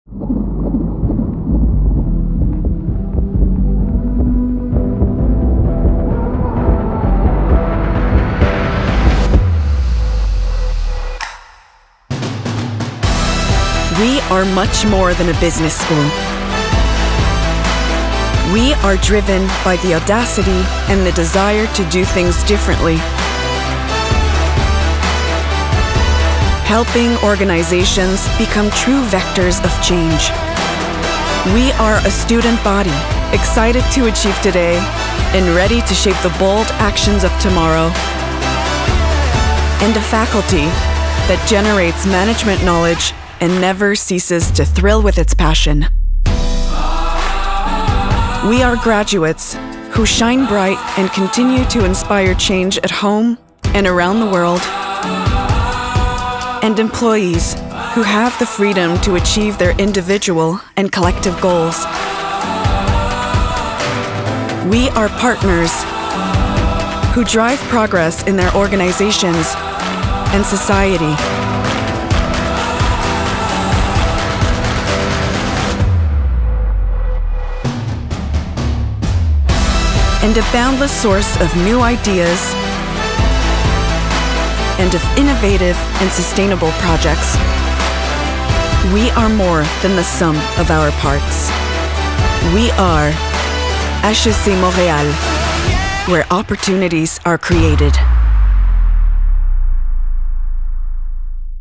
Where opportunities are created | HEC Montréal
🎙️ So many opportunities to grow, to dare to be different, to help organizations become true vectors of change.
hecmontreal: where opportunities are created.

Find out more 👉 hec.ca